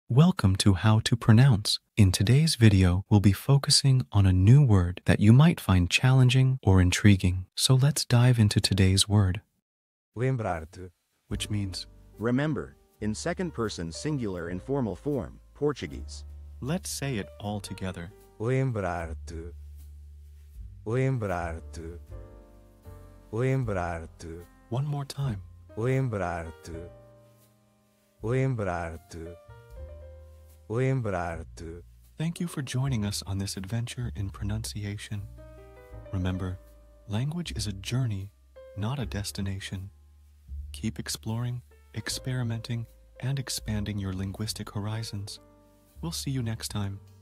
How to pronounce Lembrar-te
Master the Pronunciation of 'Lembrar-te WHICH MEANS REMEMBER YOU' - which means : "REMEMBER" INFORMAL INFORMAL Form, Portuguese. 📝 with @HTPronounce 🎓🗣️ - [Your Guide to Portuguese]

Learn to pronounce 'Lembrar-te' perfectly in Portuguese with our easy and engaging tutorial by @HTPronounce! 📖📽️ In this comprehensive video, we provide you with native speaker examples 🗣️👂 and phonetic breakdowns 🎵 to ensure you get the pronunciation just right.

How to say Remind you of in Portuguese